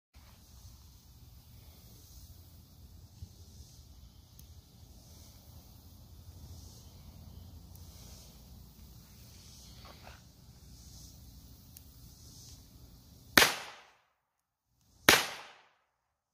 Grandpa's Old Trapper 7 shot 22 LR revolver epic demo
OLD  7 shot H&R Trapper with hexagon barrel